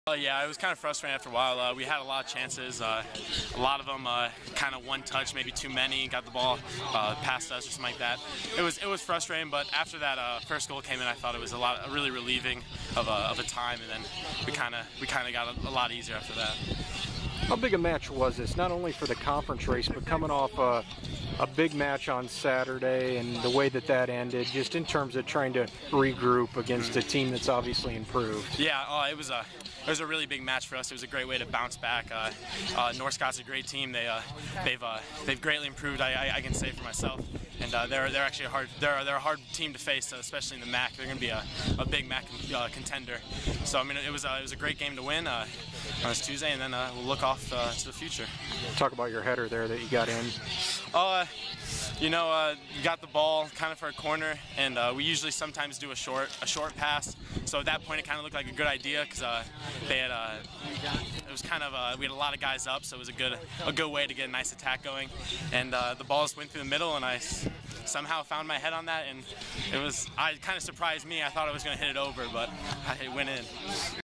Bettendorf's Tyler Dunlavy
Bettendorf senior Tyler Dunlavy had a goal and two assists Tuesday night in a 4-1 triumph over North Scott at Bettendorf High School.